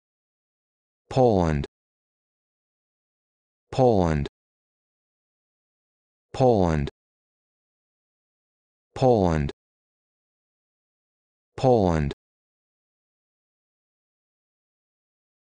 How to Pronounce Poland
This video shows you how to pronounce Poland